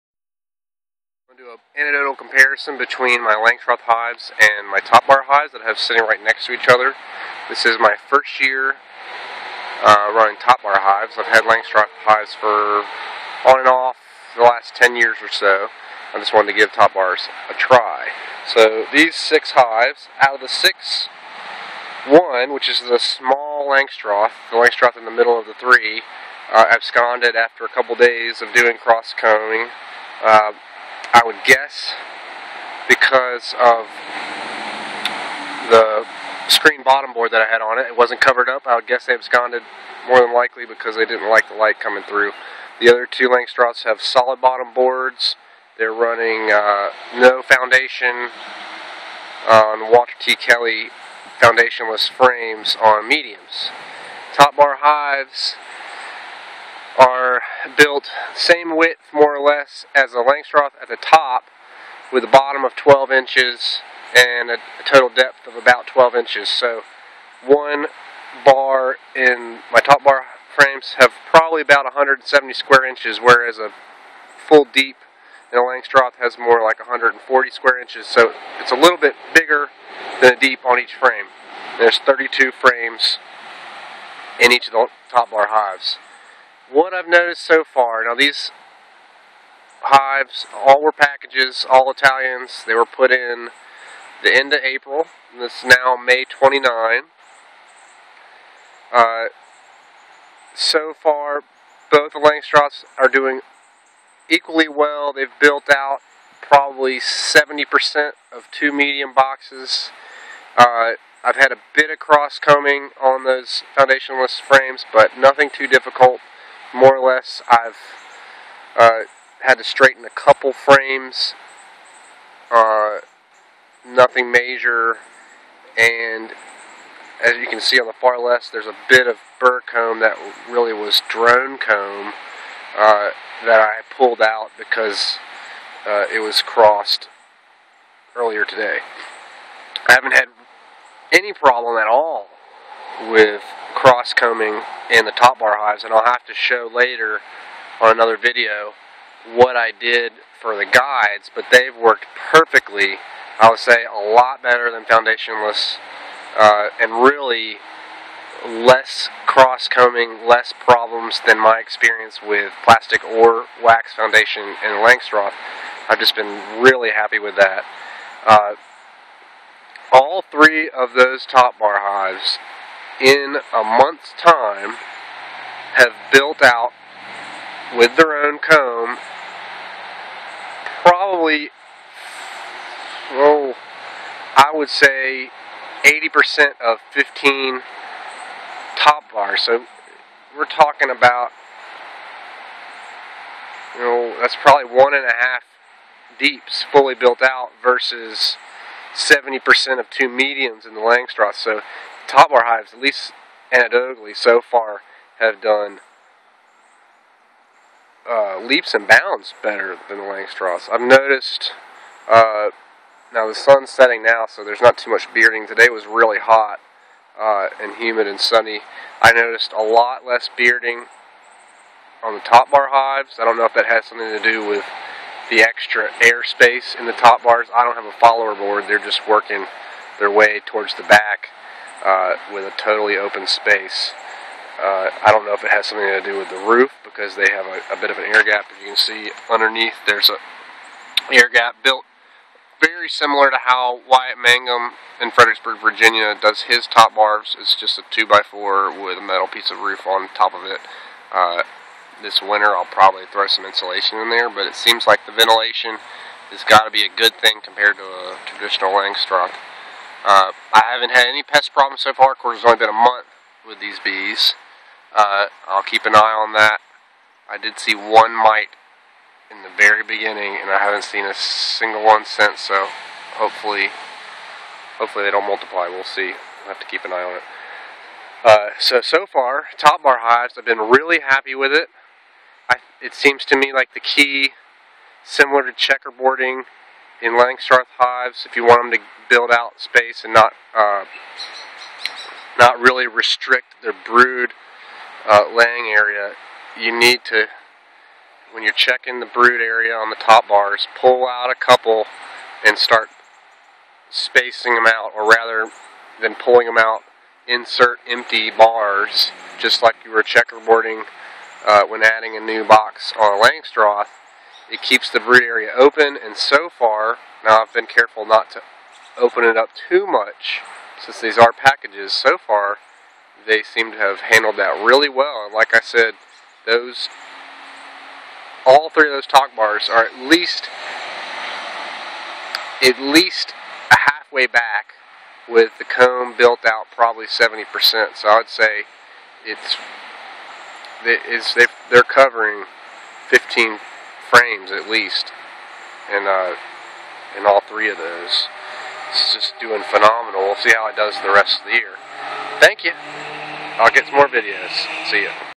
Langstroth vs. Topbar - Part 1, May 29, 2013
My initial observations and comparisons between Langstroth and Topbar beehives. All hives were started from Italian packages on April 21, 2013. All of the successful hives have shown the same calm temperament, and all have good laying patterns. The Langstroth hives are 10-frame mediums, with foundation-less frames, standard tops and inner covers, and solid bottom boards. The Top-bar hives each contain 32 1-3/8" bars, each approximately 30% larger than a Langstroth deep frame (each topbar hive has a total volume close to four 10-frame deeps). Top-bar hives are all equal dimensions and with solid bottom boards. All packages were fed a 2:1 sugar syrup until they had capped stores visible, which was 3 weeks after install and about 2 gallons total.